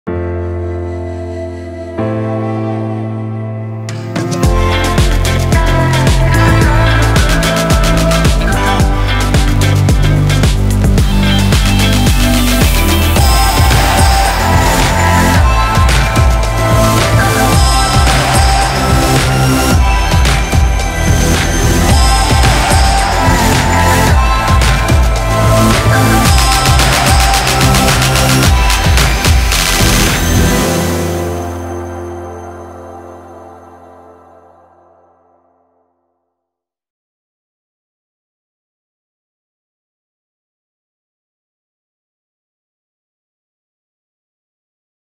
Eve Energy (US) - Endless possibilities
With Eve Energy, switch your devices on or off using Siri or with a simple tap, and instantly see how much energy your devices are using.